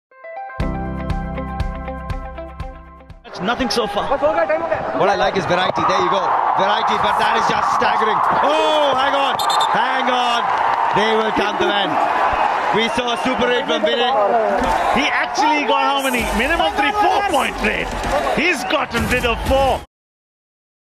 K Prapanjan's Super Raid vs Haryana Steelers
4⃣ points bhi churaya, hamara dil bhi! 😉

K. Prapanjan on the prowl spells D A N G E R! ☠